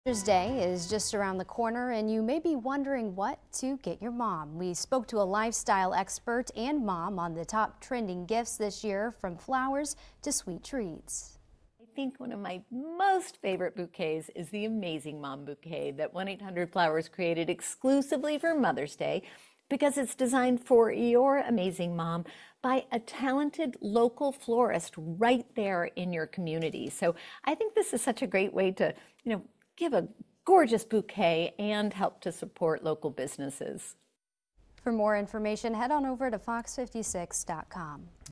Mother's Day gifts: support local florists and indulge in sweet treats
Mother’s Day is just around the corner..

And you may be wondering what to get your mom.

We spoke to a lifestyle expert and mom on the top trending gifts this year from flowers to sweet treats.